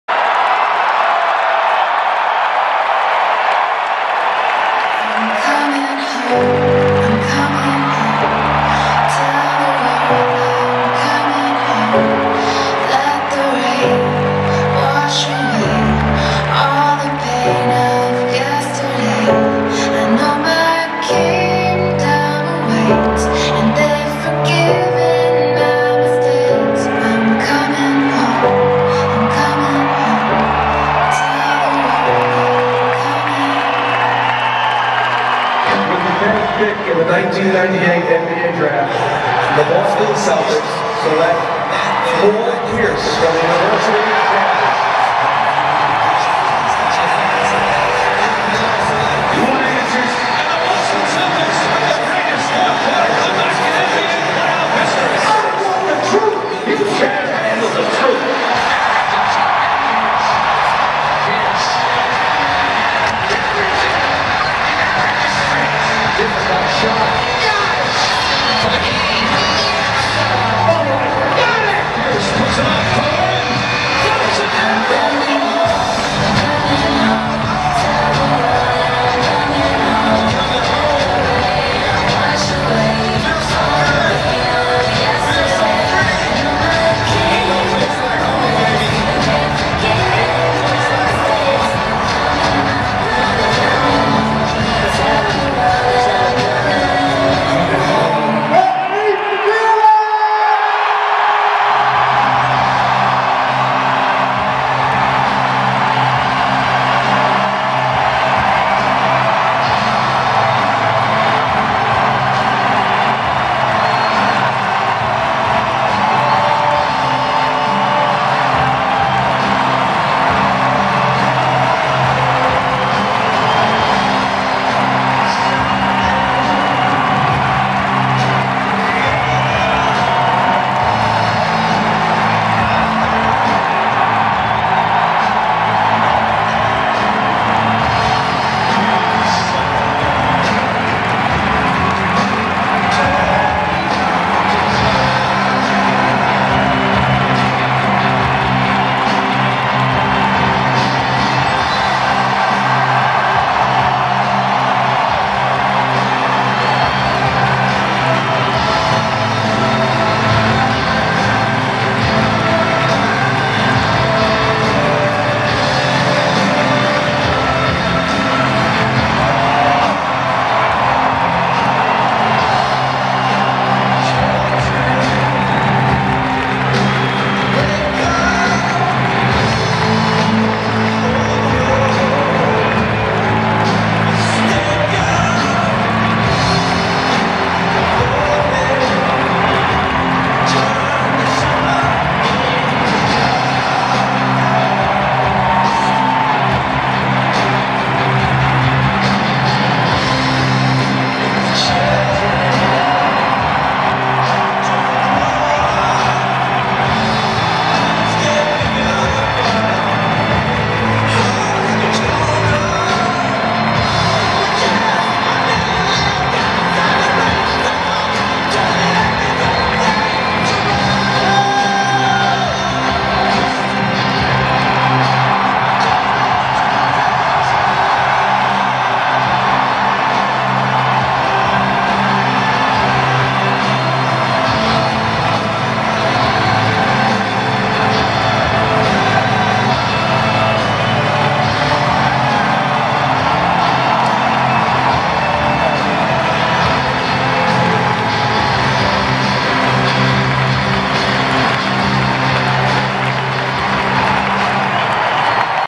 'Thank you, Paul': Paul Pierce Celtics video tribute
BOSTON -- The Celtics pay video tribute to former captain Paul Pierce during his first game back in Boston since being traded to the Brooklyn Nets in 2013. Pierce is second in franchise history only to John Havlicek in scoring.